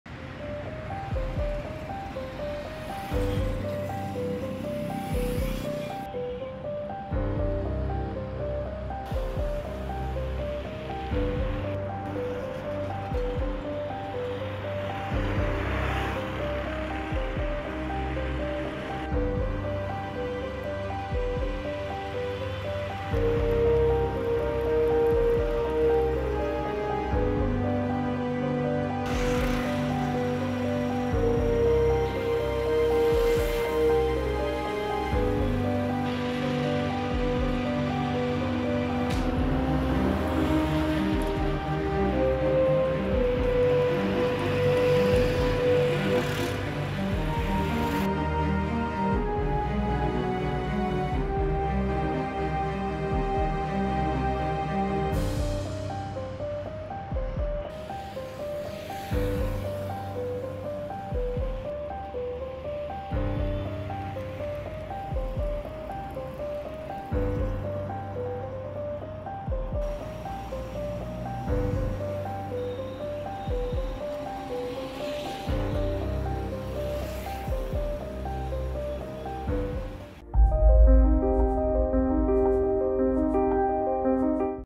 Farmers' Protest: Heavy Police Force Deployed at Ghazipur border | India TV English News
Farmers' Protest: Heavy Police Force Deployed at Ghazipur border. Farmers from Haryana have called the ‘Delhi Chalo’ march to press for their demands. Watch the video to know more!